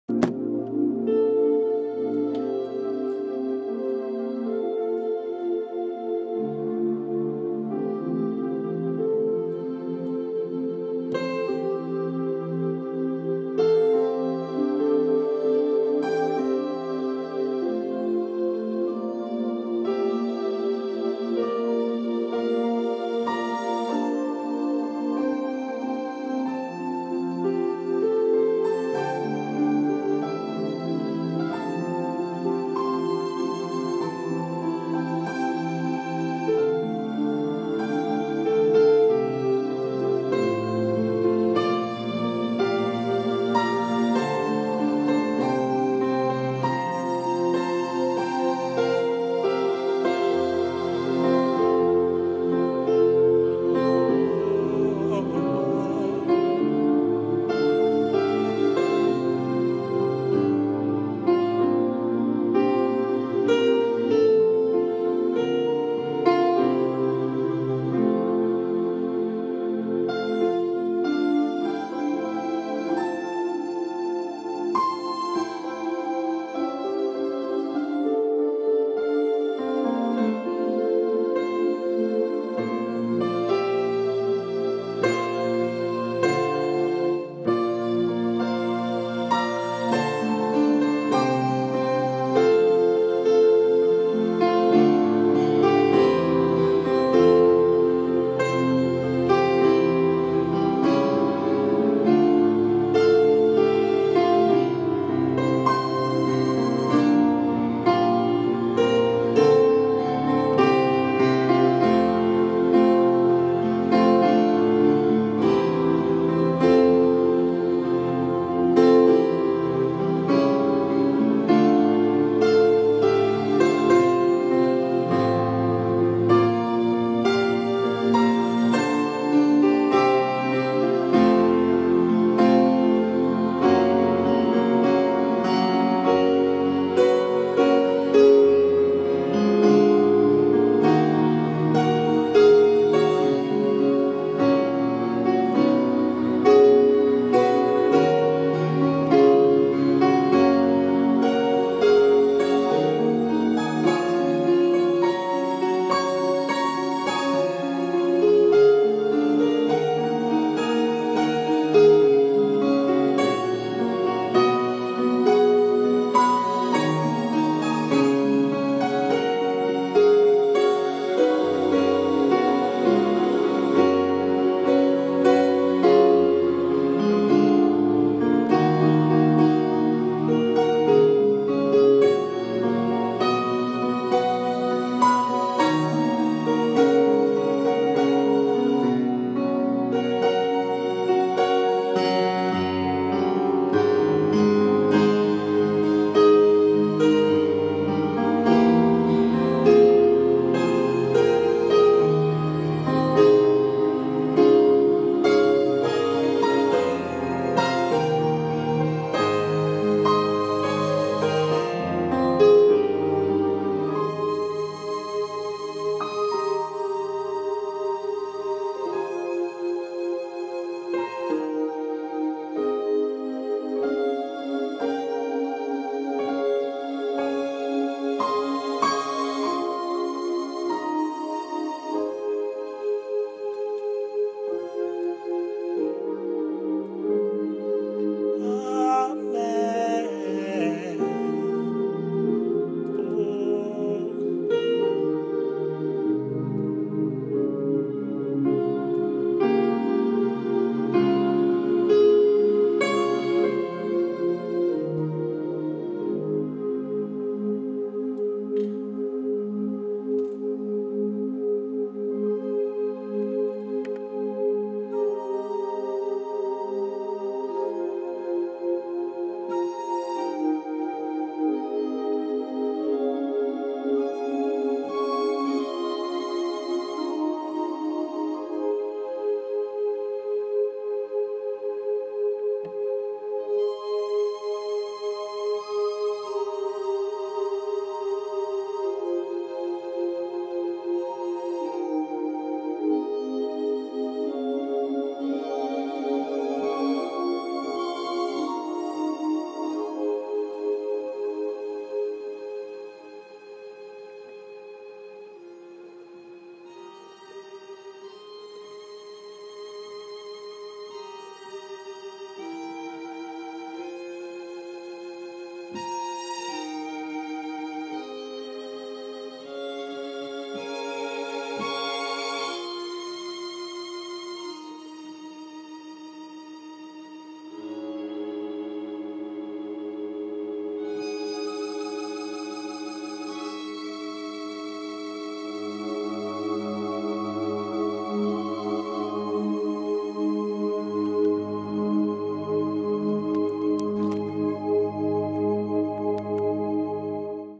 Worship
Just recored one of my worship sessions one day. Be Blessed